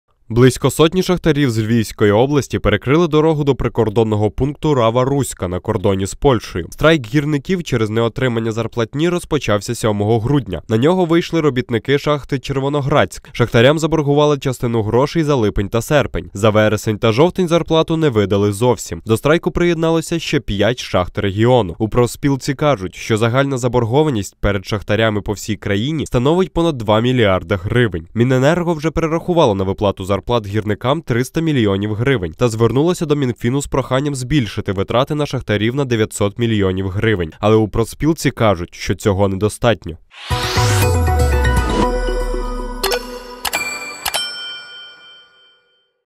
Страйк шахтарів через борги по зарплаті
Близько сотні шахтарів з Львівської області перекрили дорогу до прикордонного пункту "Рава-Руська" на кордоні з Польщею.

Страйк гірників, через неотримання зарплатні, розпочався 7 грудня - на нього вийшли робітники шахти "Червоноградська". Шахтарям заборгували частину грошей за липень та серпень, за вересень та жовтень зарплату не видали зовсім. До страйку приєдналося ще п'ять шахт регіону.

У профспілці кажуть, що загальна заборгованість перед шахтами по всій країні становить понад 2 млрд гривень. Міненерго вже перерахувало на виплату зарплат гірникам 300 млн гривень та звернулося до Мінфіну з проханням збільшити витрати на шахтарів на 900 млн гривень. Але у профспілці кажуть, що цього недостатньо.